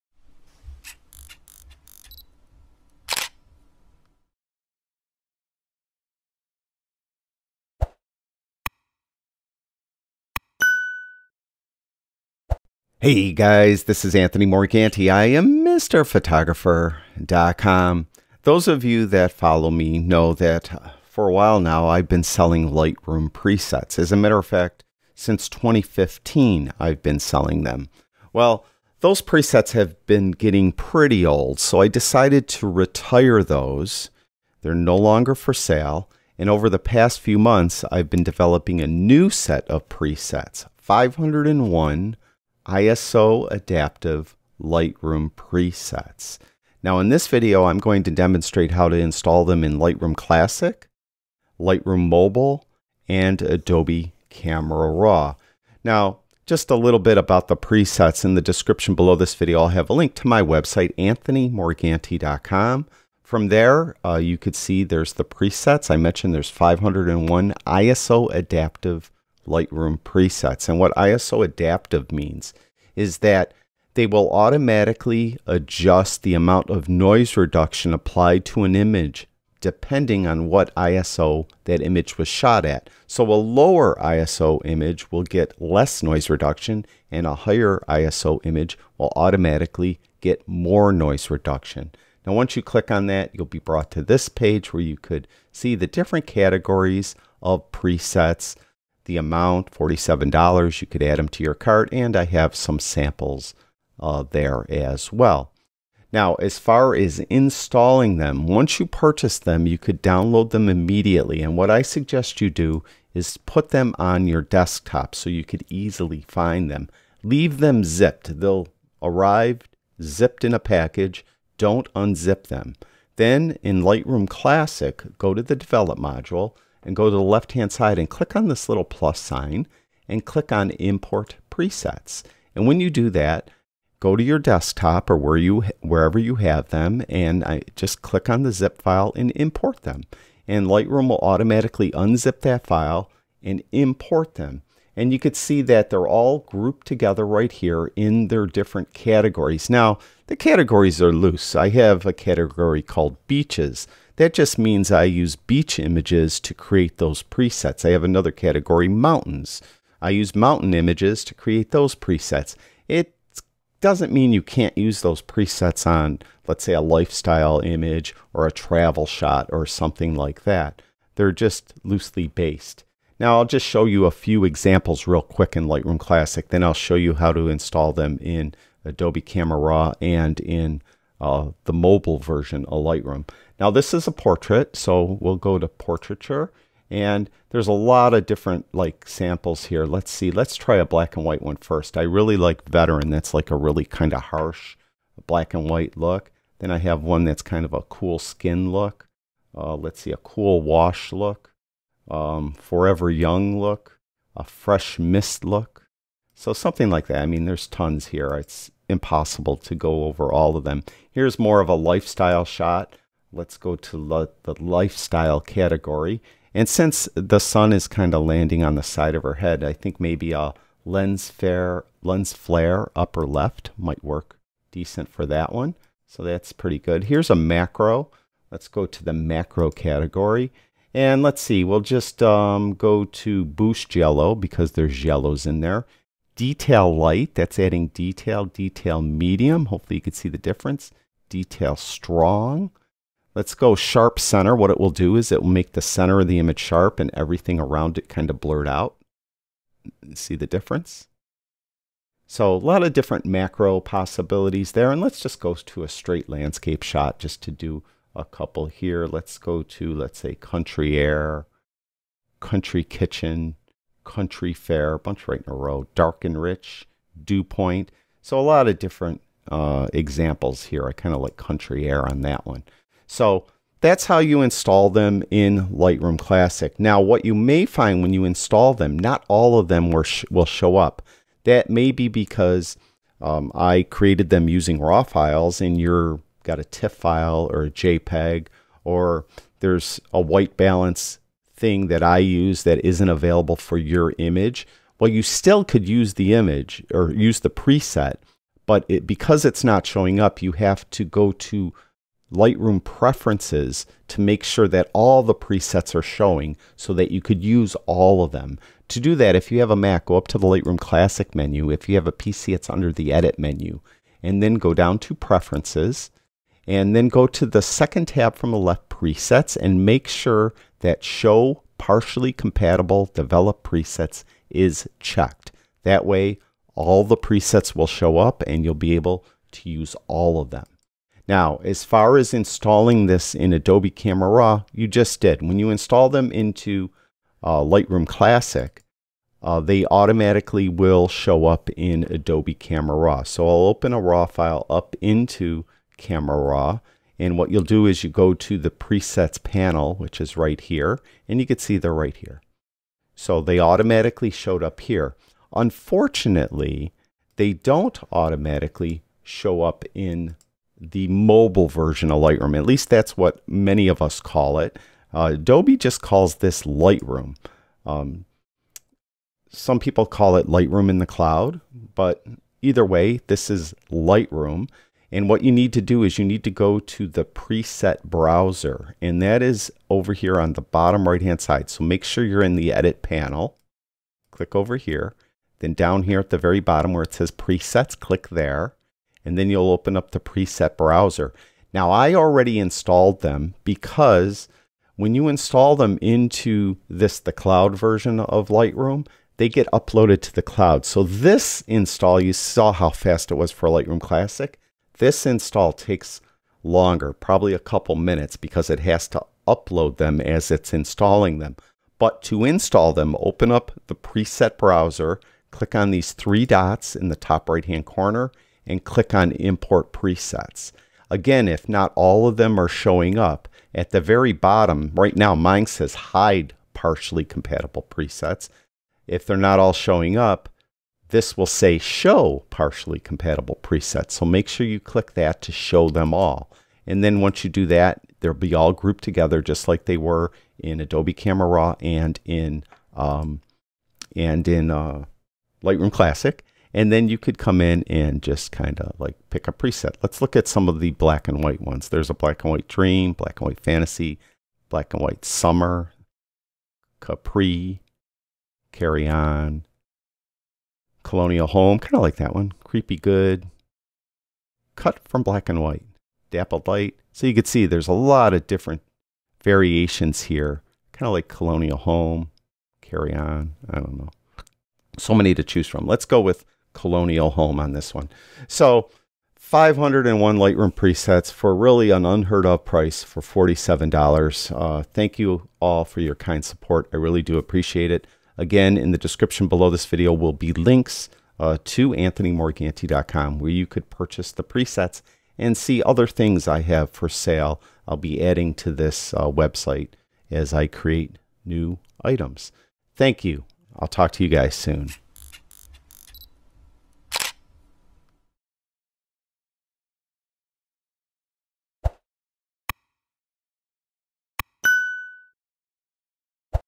501 Lightroom Presets - $47
In this video, I demonstrate how to install my brand new package of Lightroom Presets in Lightroom Classic, Lightroom Mobile, and Adobe Camera RAW.